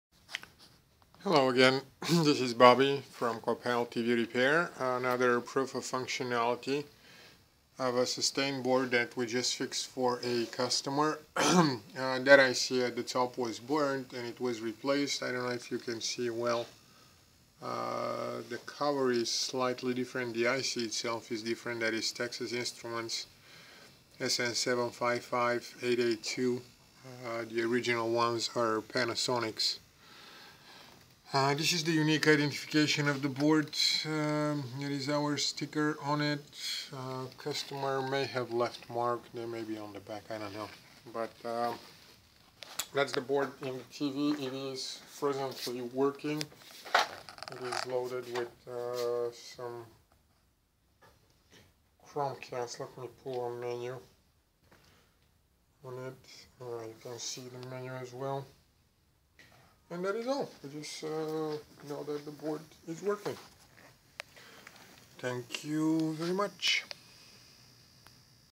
Samsung PN50B450 Y-Main LJ92-01601A proof of functionality F.Munoz
This is a proof of functionality of Y-Main board  LJ92-01601A for Samsung PN50B450 after we've replaced a burned output register IC and before we return it to customer.